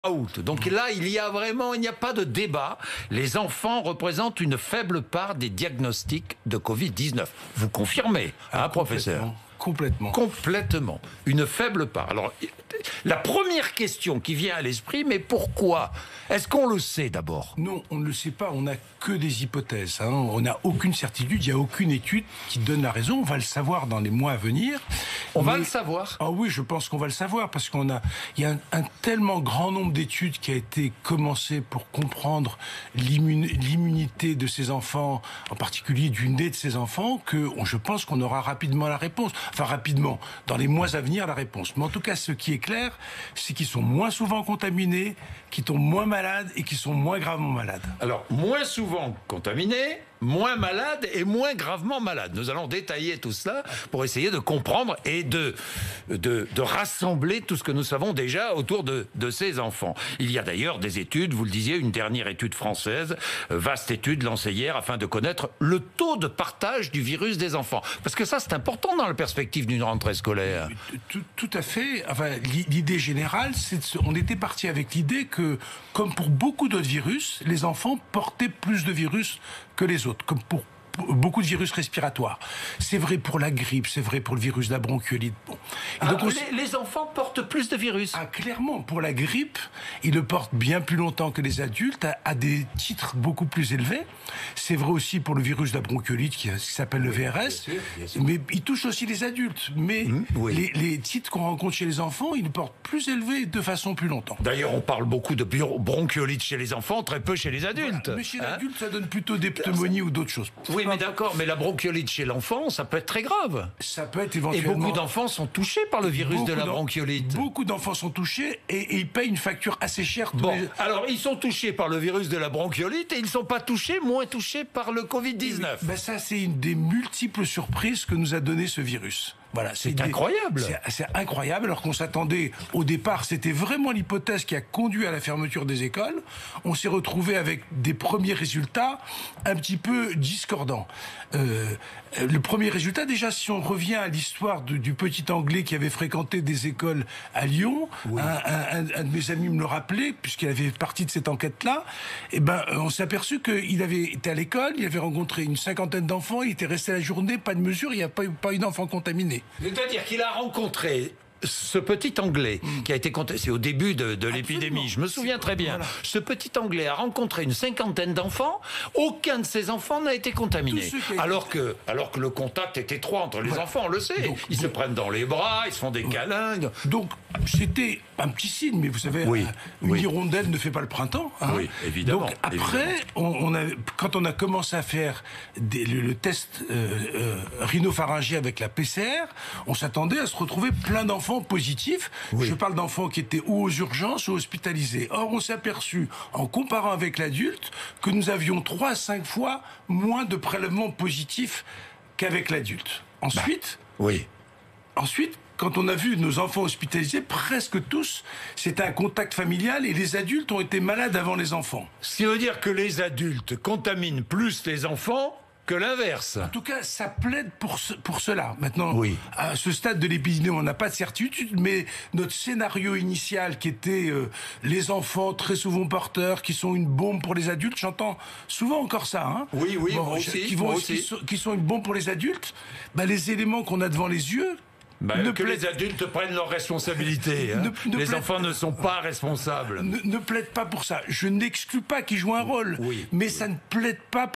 LEs enfants "Ils sont moins souvent contaminés, moins malades, moins gravement malades"
Les enfants, une faible part des malades du Covid-19?
"On a des hypothèses (...) On a 3 à 5 fois moins de prélèvements positifs qu'avec l'adulte" indique Robert Cohen, pédiatre infectiologue